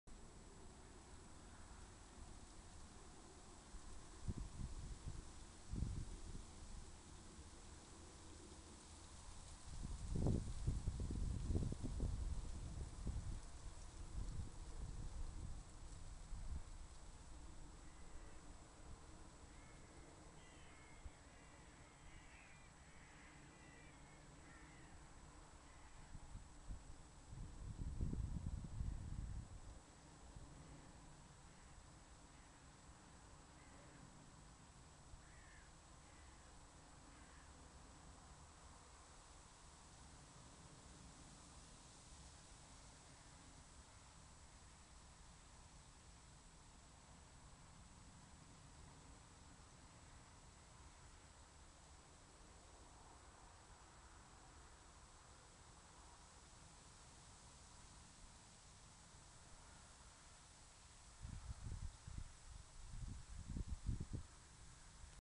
Ballet de Poissons Chats (IgaBiwa) 04 Septembre 2013
Groupes de petits poissons chats